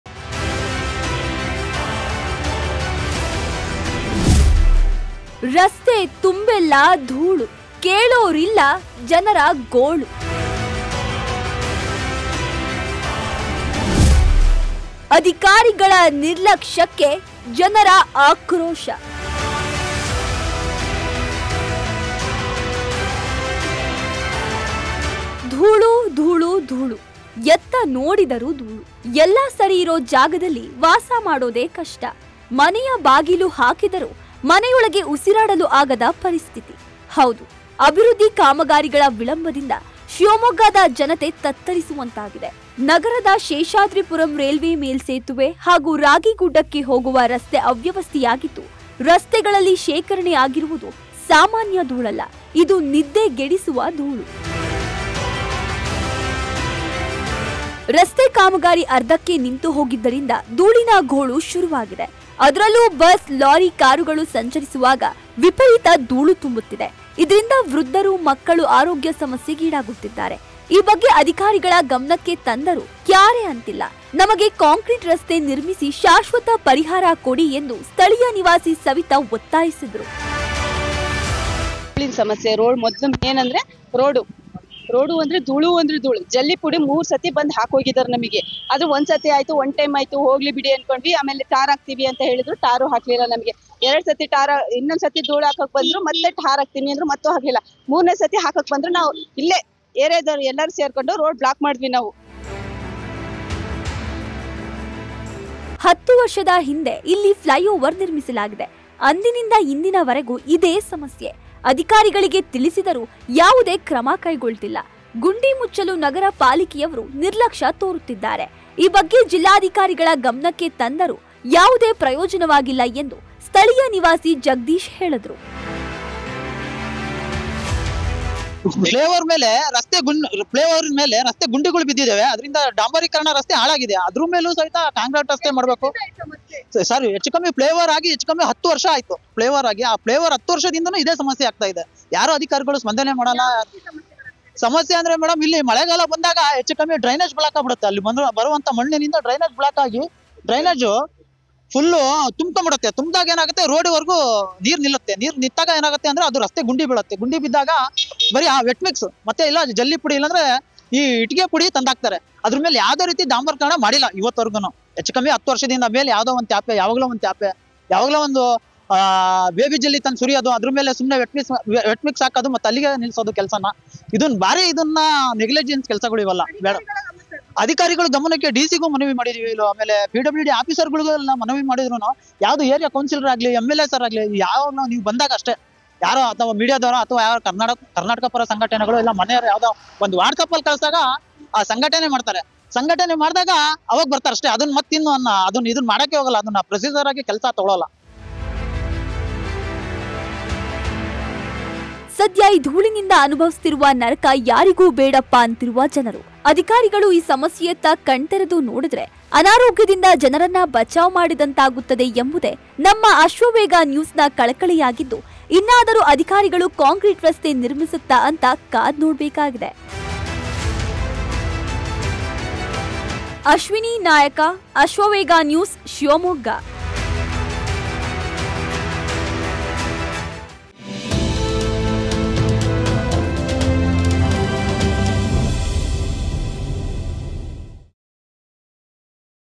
Shivamogga || ರಸ್ತೆ ತುಂಬೆಲ್ಲಾ ಧೂಳು, ಕೇಳೋರಿಲ್ಲ ಜನರ ಗೋಳು
🌪️ Shivamogga: Dust Everywhere, People Struggling for a Solution! 🌪️